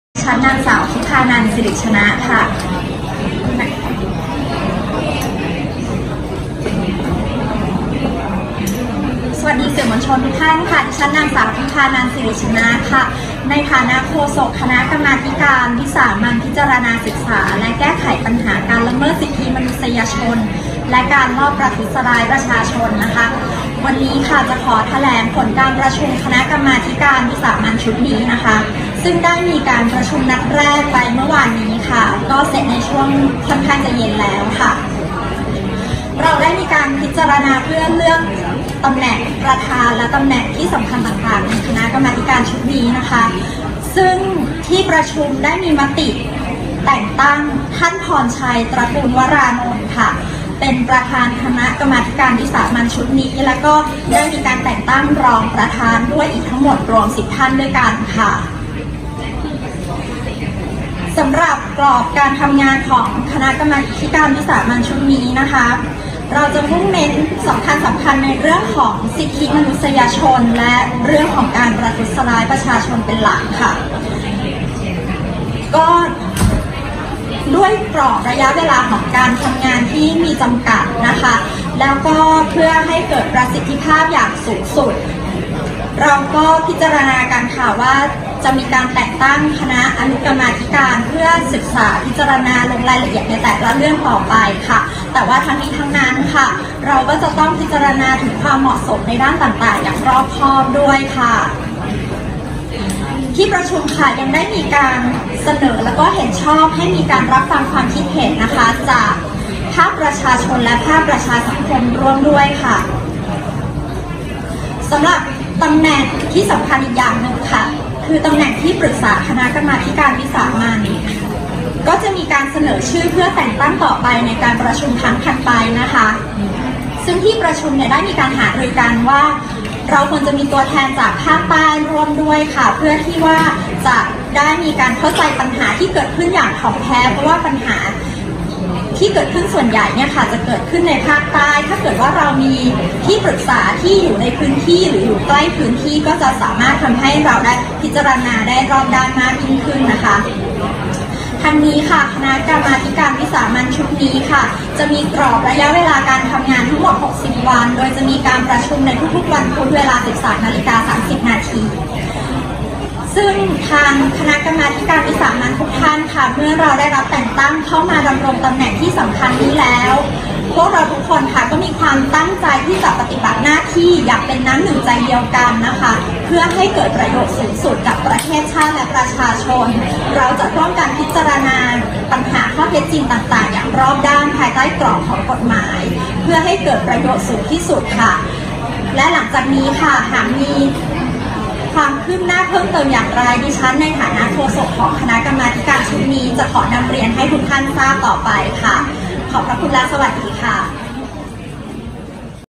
“ทิพานัน” เผย กมธ.วิสามัญศึกษาและแก้ไขปัญหาการละเมิดสิทธิมนุษยชนฯ เลือก “พรชัย”
26/12/2019 “ทิพานัน” เผย กมธ.วิสามัญศึกษาและแก้ไขปัญหาการละเมิดสิทธิมนุษยชนฯ เลือก “พรชัย”
“ทิพานัน” เผย กมธ.วิสามัญศึกษาและแก้ไขปัญหาการละเมิดสิทธิมนุษยชนฯ เลือก “พรชัย” เป็นประธาน พร้อมตั้งกรอบการทำมุ่งเน้นประโยชน์ของประชาชน